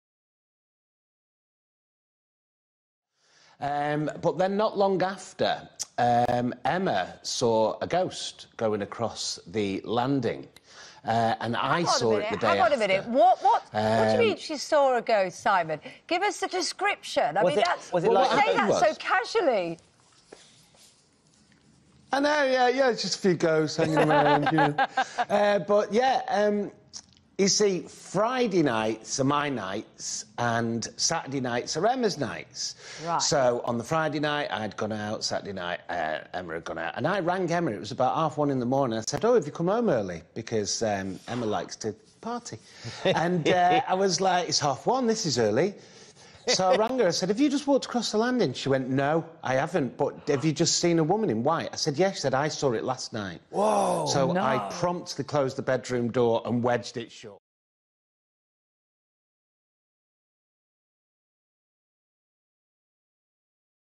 Corrie Actor Simon Gregson Calls In Ghost Hunters To Deal With Woman In White
Actor Simon Gregson who plays Steve McDonald on Coronation Street reportedly barricaded himself in a room of his home after seeing a ghost while home alone.

Copyright is not owned by this channel unless otherwise stated.